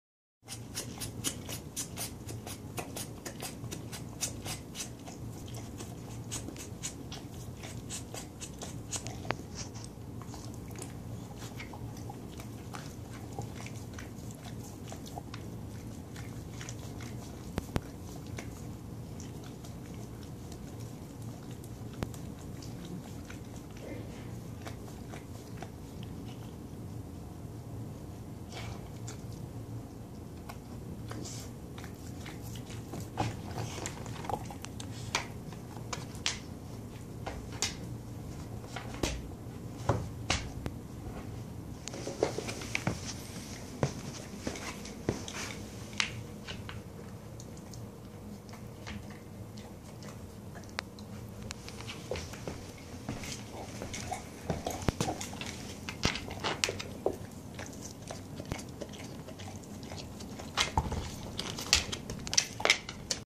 He loves the Kong with peanut butter
Going at it at my studio with the King filled with peanut butter.